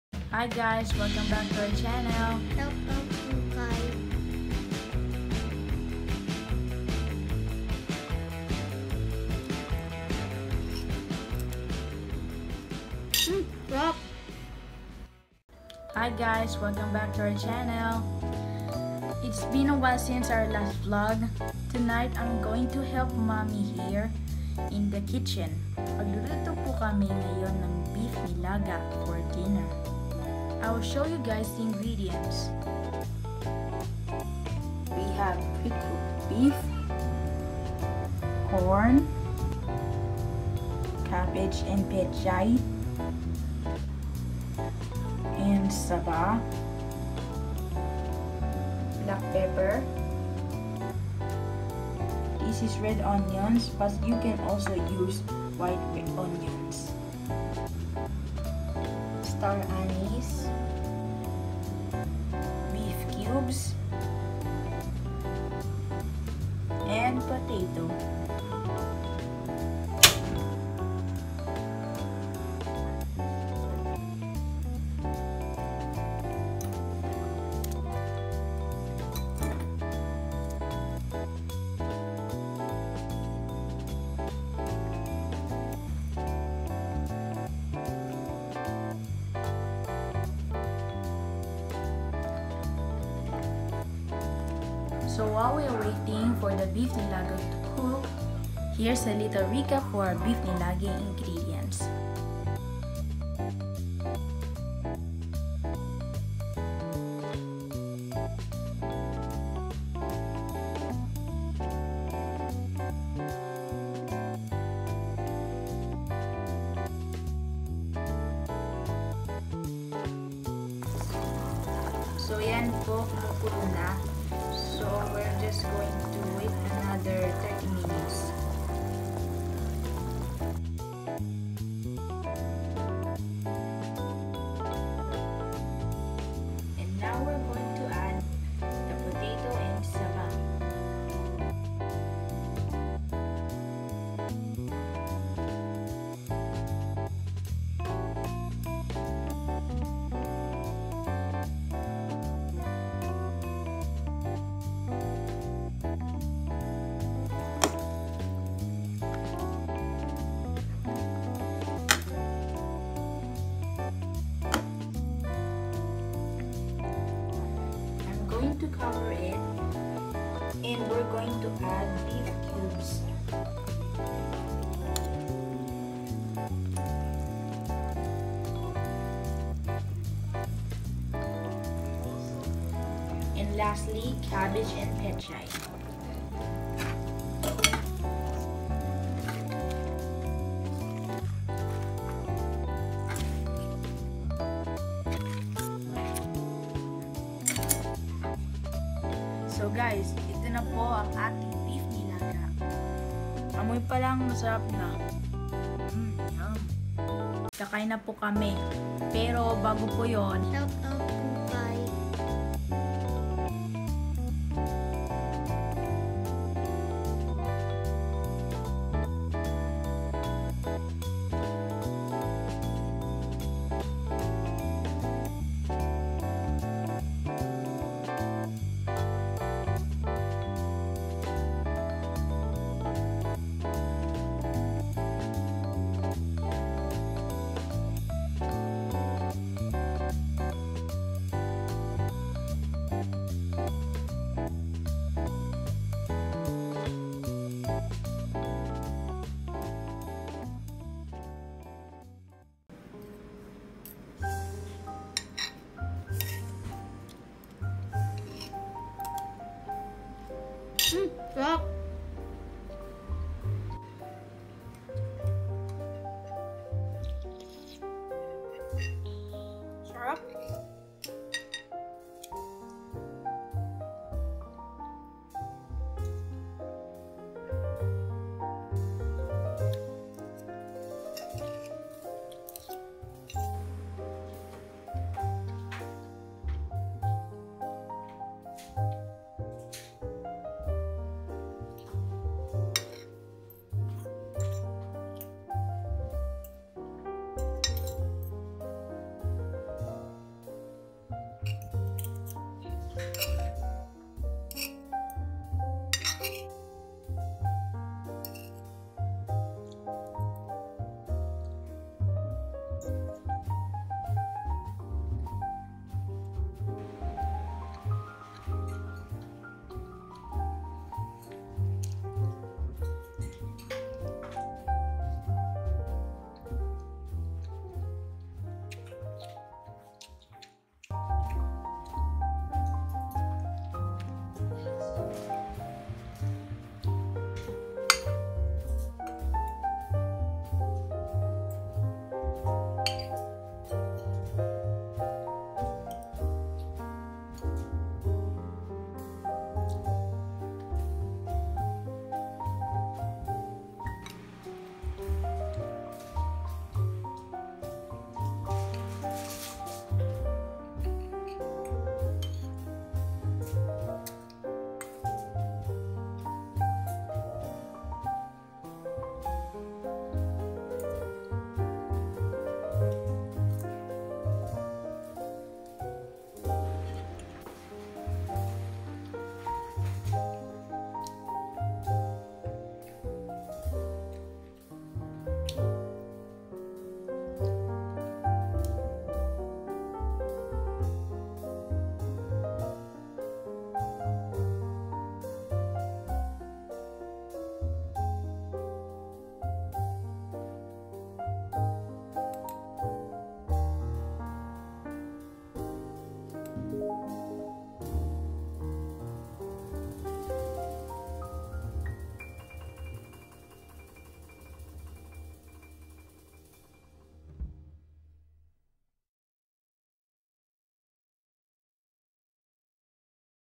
Nilagang Baka | Pinoy Dish | Luto ni Kuya | shout out
BEEF NILAGA is a Filipino style boiled soup with fork tender beef shanks and vegetables. The heart of this dish 'beef nilaga' is the broth. Beef nilaga is definitely the ultimate comfort food perfect to serve during rainy  or cold season. This meal can be enjoyed on its own but traditionally it is served with rice.  Serve hot.

Ingredients:
Beef
Corn
Cabbage
Saging saba
Pechay
Onion
Star anise
Pepper Corn
Beef cubes
Salt